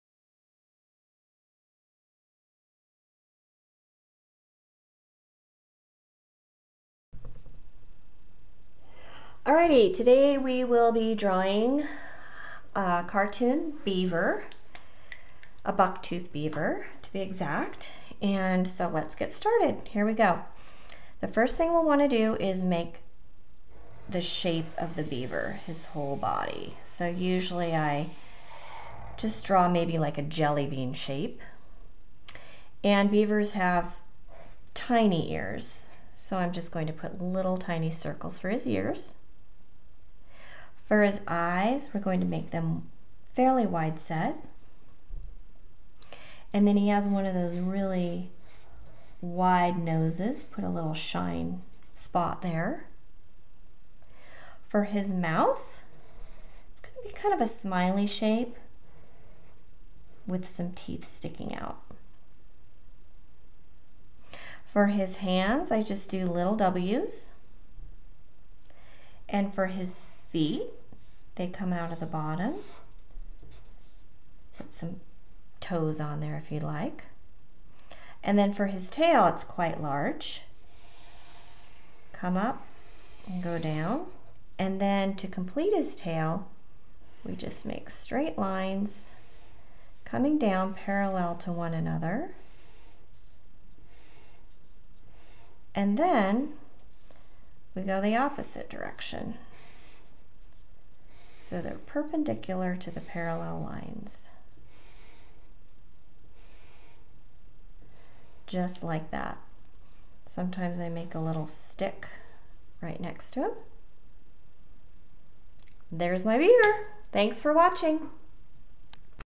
How to Draw a Beaver Cartoon Easy Drawing Tutorial
Watch this video to learn how to draw a Beaver! This video shows how easy it is for beginners to follow along and create a simple and cute drawing step-by-step.

•Explore the Art of Adding Watercolor Paints to Doodle Drawings on Etsy: